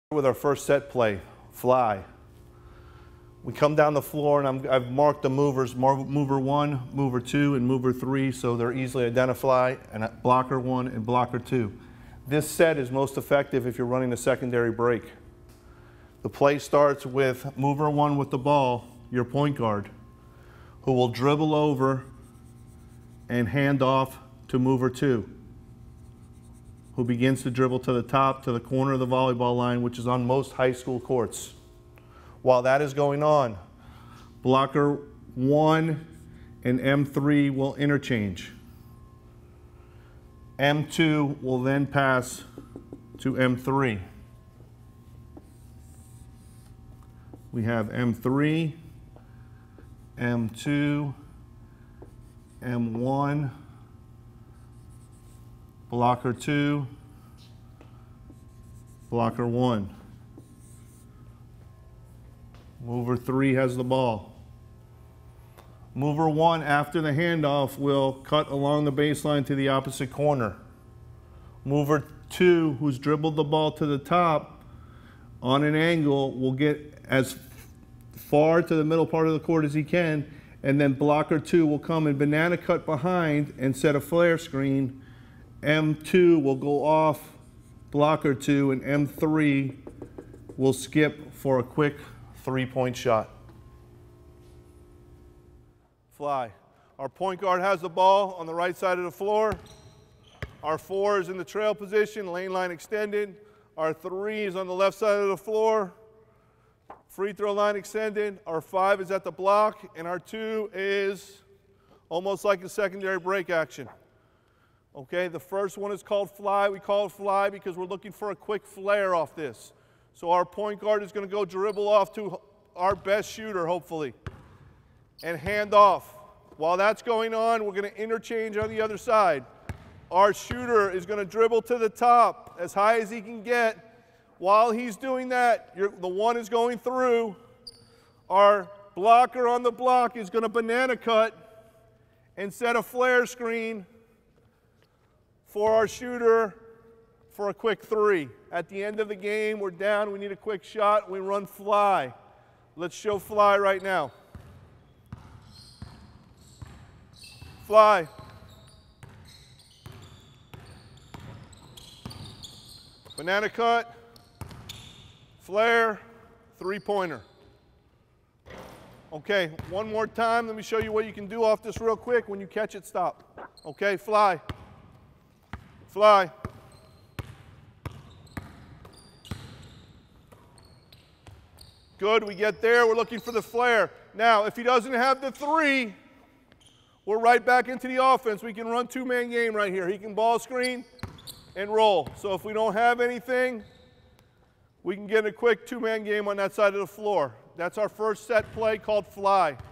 Mover-Blocker Set Play for a Three! - Basketball 2017 #12
John A. Ferguson High School (Florida) head coach, Tom Jicha, diagrams and runs through his “Fly” set for the Mover-Blocker offense. The set is designed to get your team an open 3-pointer and is most effective if you are running a secondary break.